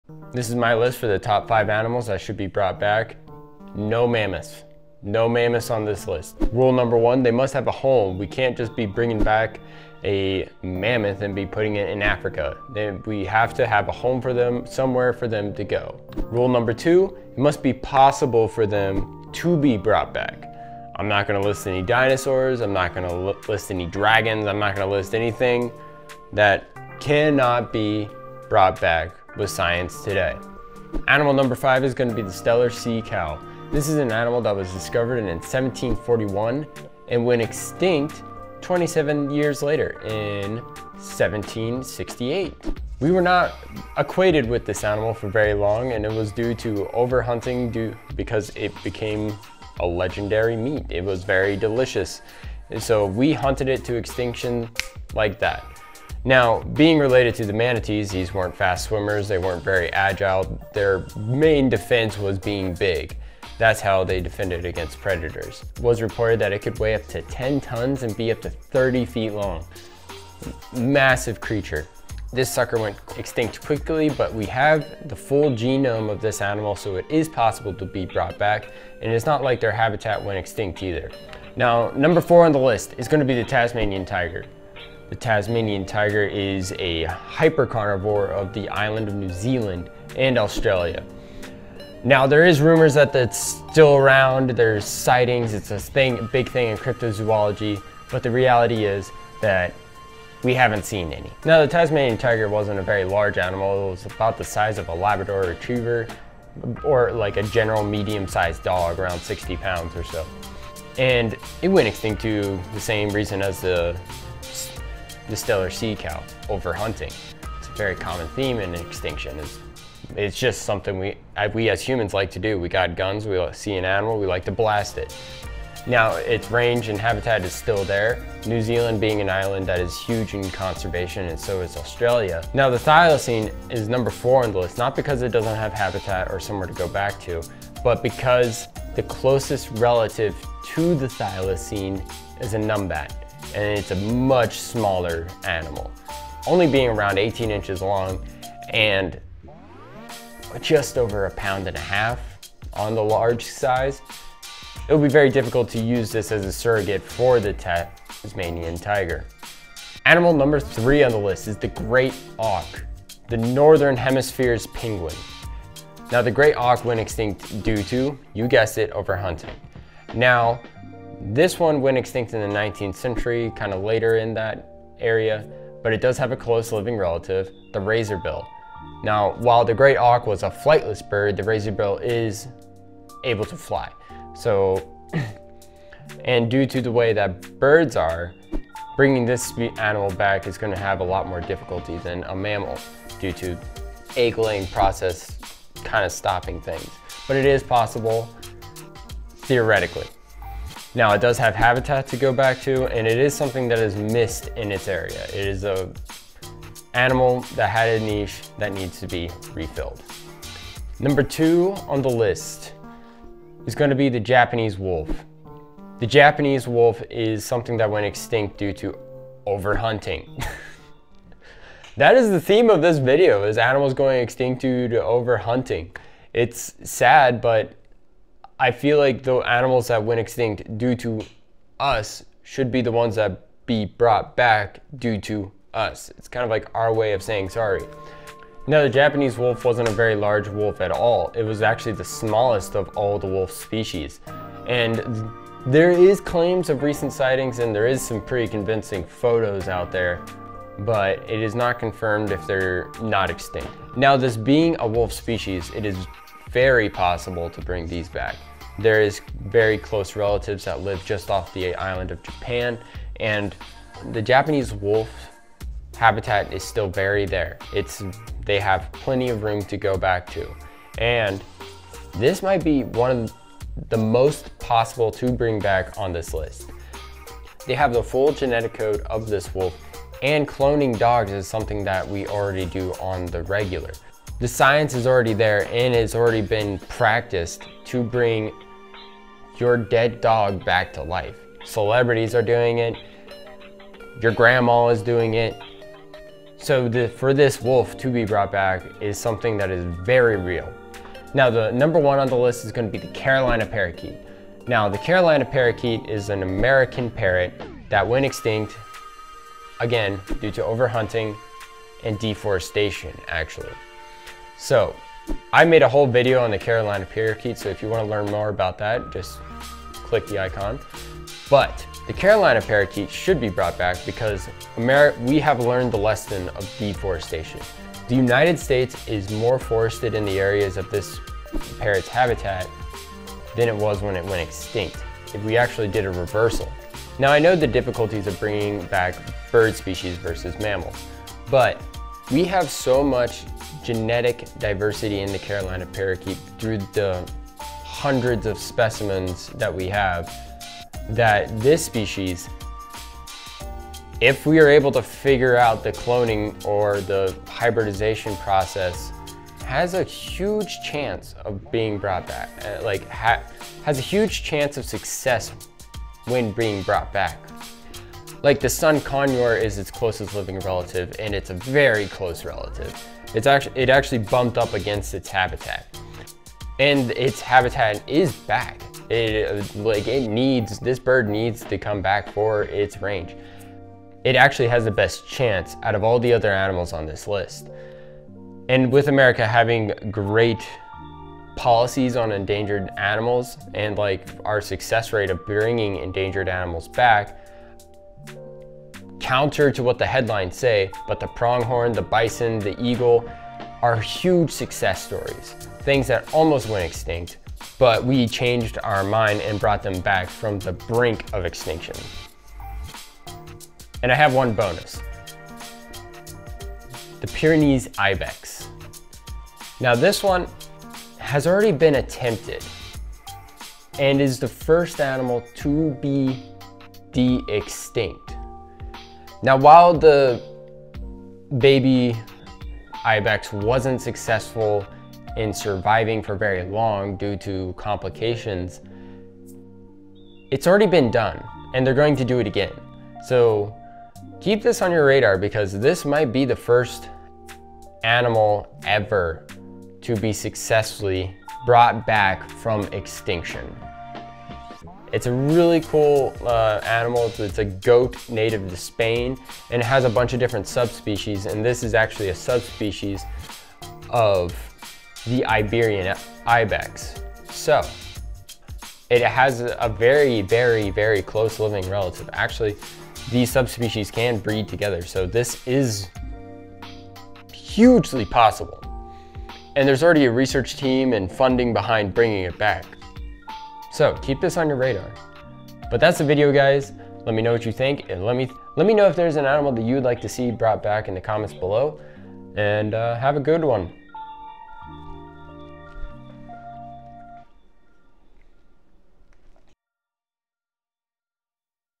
Top 5 Animals We Should Bring Back From Extinction
This is my list of animals that are not only possible, but should be brought back from extinction. The science is there for them to be brought back, and the have habitat to return to. Things that are often let out of the conversation.

Chapters
0:00 - intro
0:30 - Animal number 5
1:21 - Animal number 4
2:40 - Animal number 3
3:31 - Animal number 2
5:01 - Animal Number 1
6:57 - Bonus